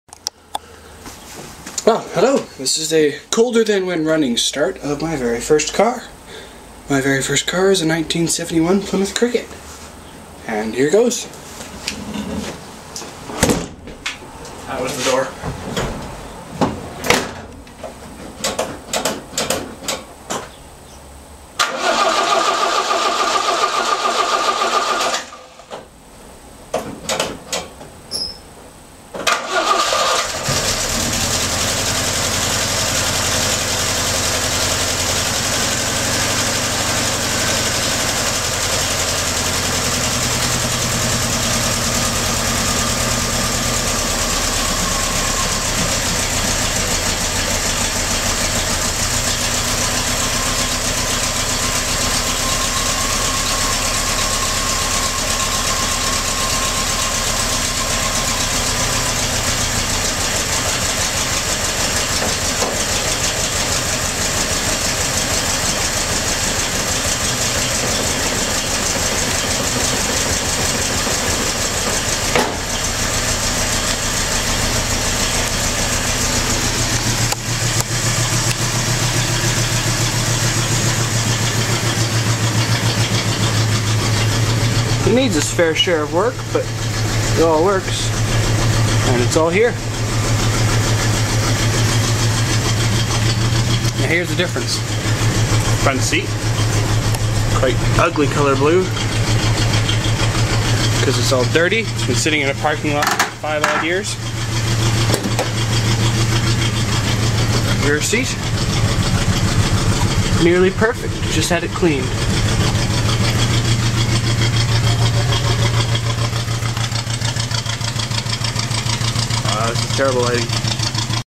New cold start of 71 Plymouth Cricket
I just discovered that I had this film (completely forgot taking it). It is of the car when I had just gotten it, and was in the process of cleaning it, nothing more.